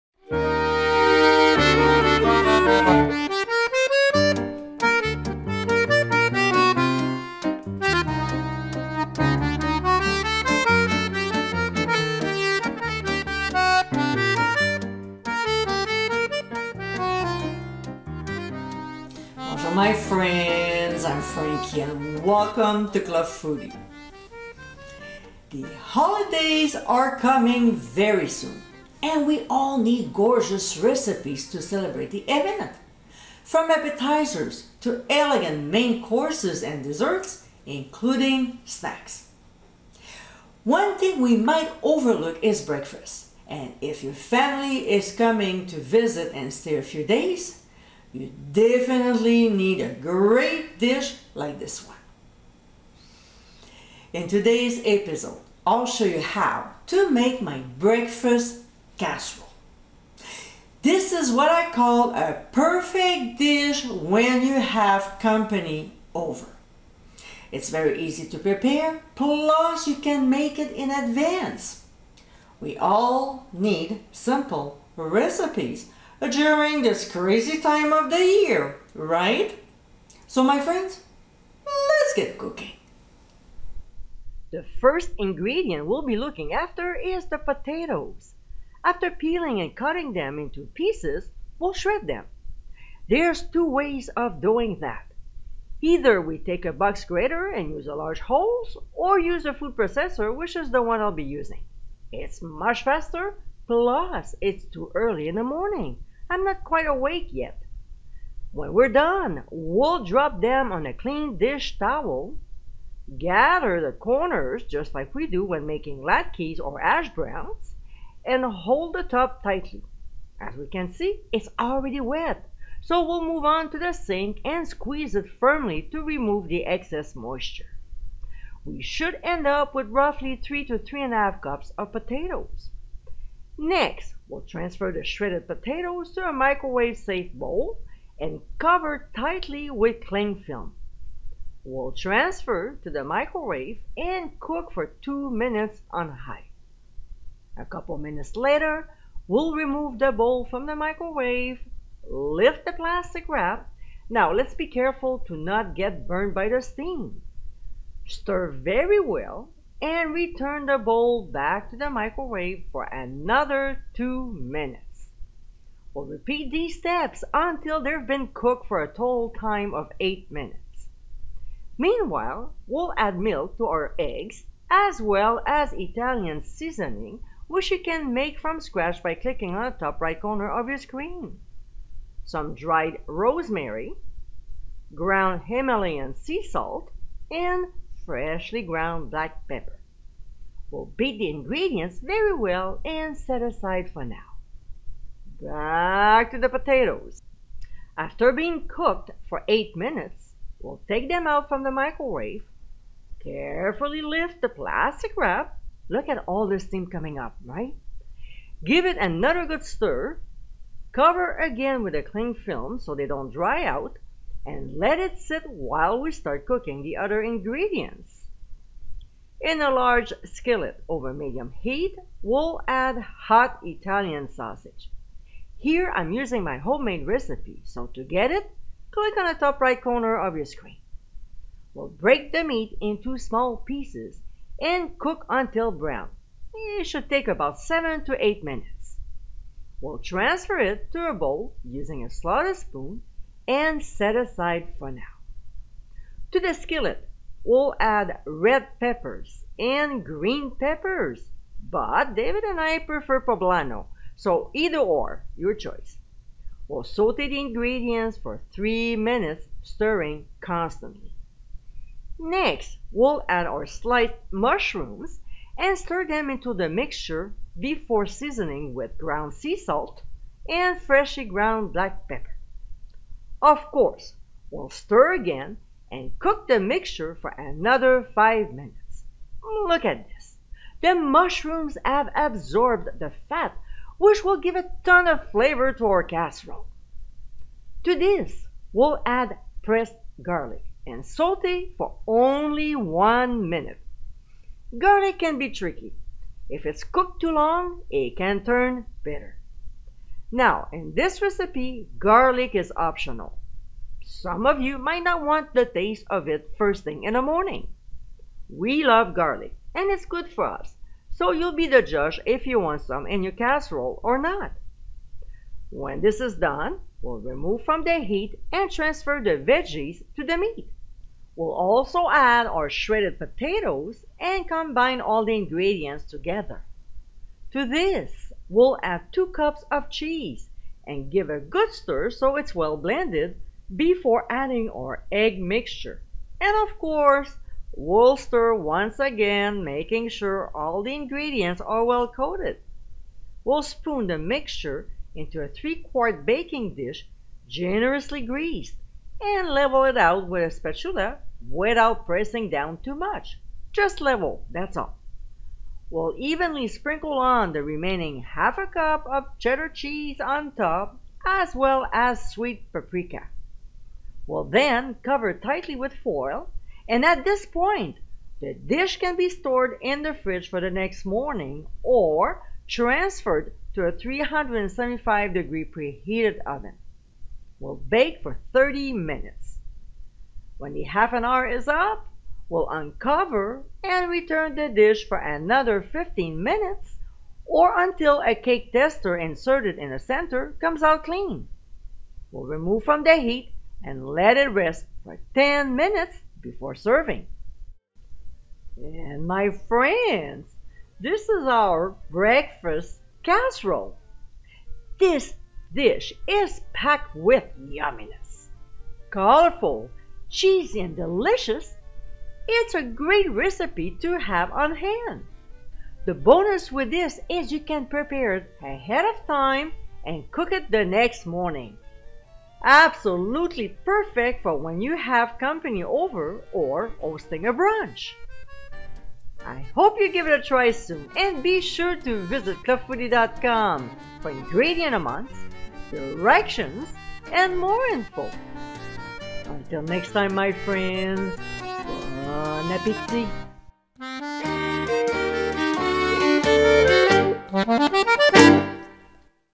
Breakfast Casserole Recipe • Great Morning Starter! 🌞 - Episode 582
Bonjour my friends! I’m Frankie and in this episode, I'll show you how to make my Breakfast Casserole recipe. 🌞

for ingredient amounts, directions, and much more!
😀 and join the Club for FREE!!!